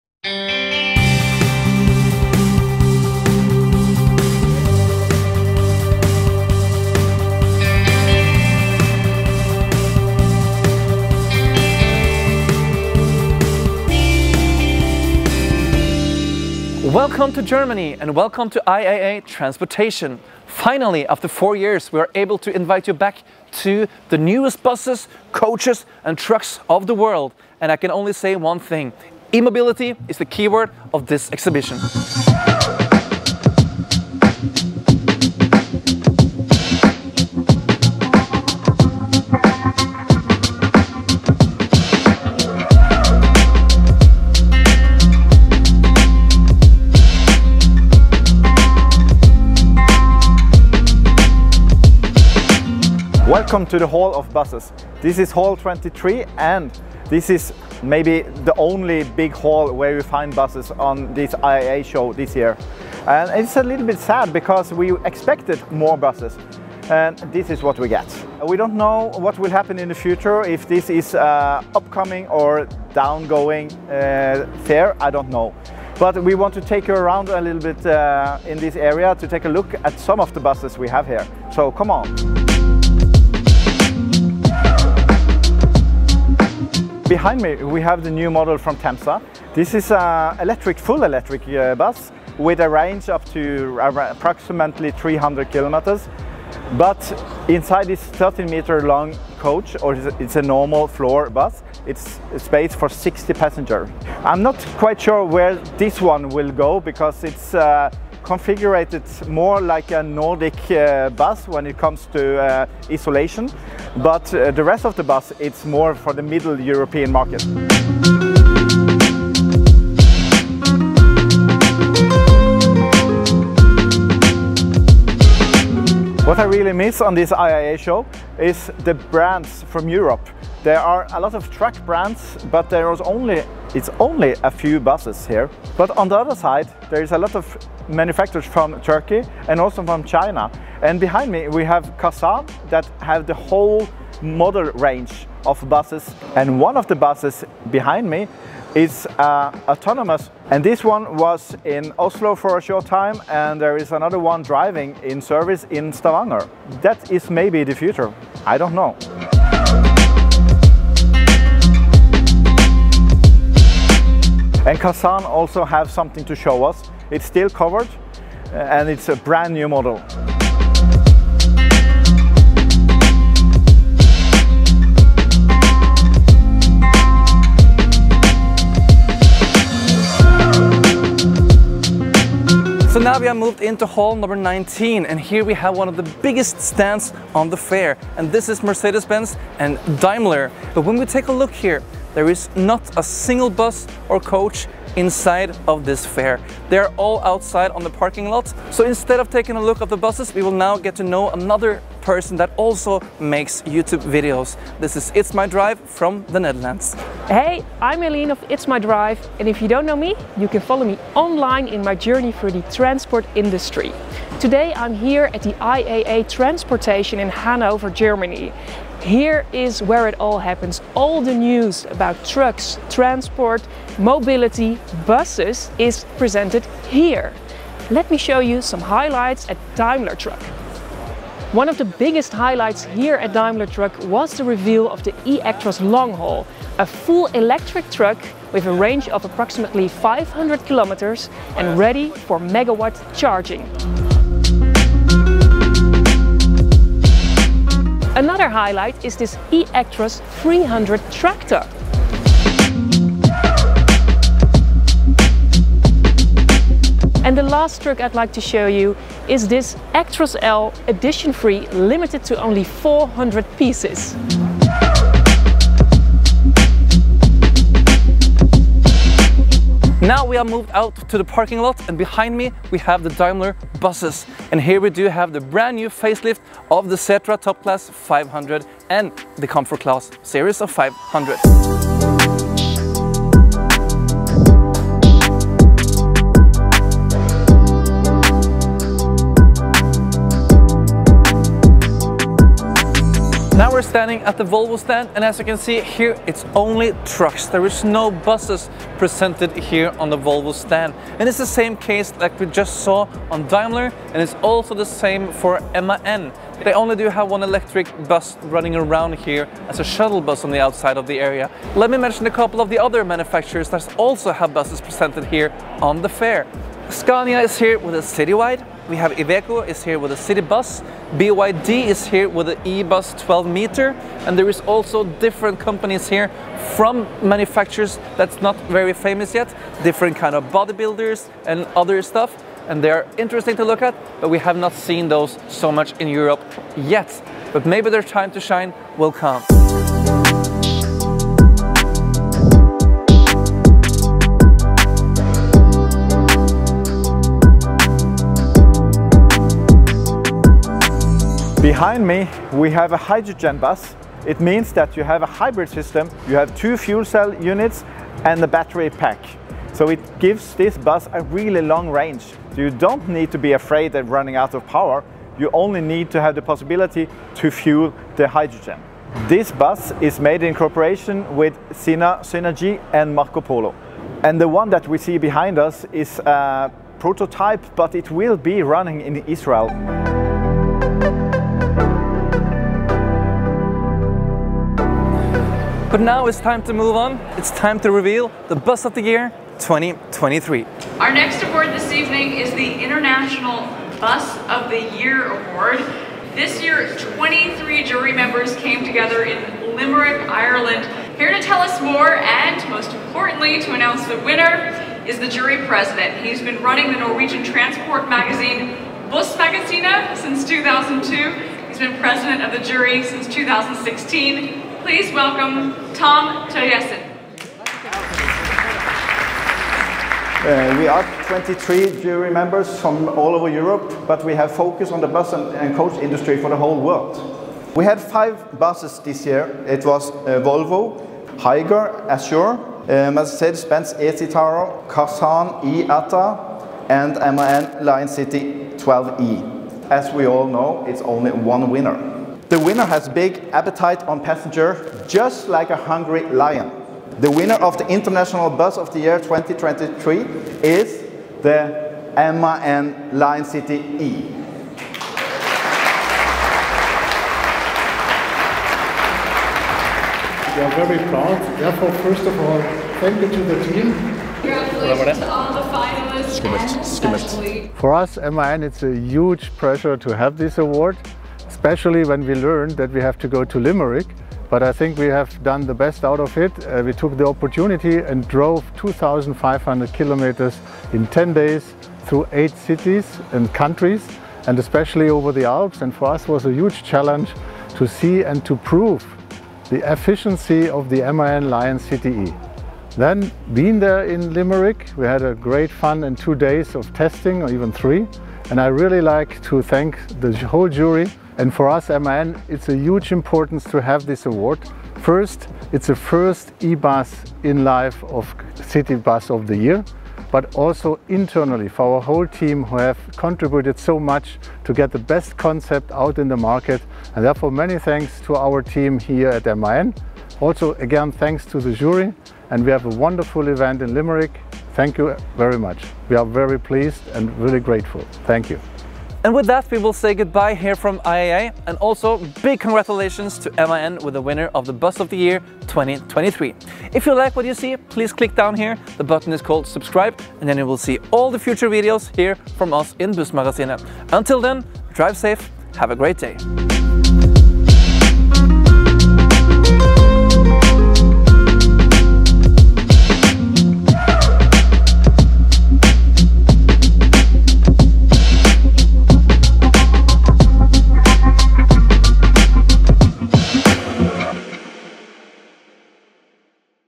IAA TRANSPORTATION 2022
Welcome to IAA Transportation 2022!

Join us in Hannover where we are taking a closer look att the newest busses, coaches and trucks on the market. Finally, after four years, the IAA is back. Welcome in!

Velkommen til IAA Transportation 2022!

Bli med til Hannover der de nyeste bussene og lastebilene ble vist frem i september. Etter fire år er messen endelig tilbake. Men, vi mangler en ting. Det er veldig lite buss på messen i år! Bli med oss inn på messen for å se på de bussene som var tilstede

BEST MUSIC FOR YOUR VIDEOS!

FILM:

► Magne Håheim
► Tom Terjesen

EDIT:

► Magne Håheim

Web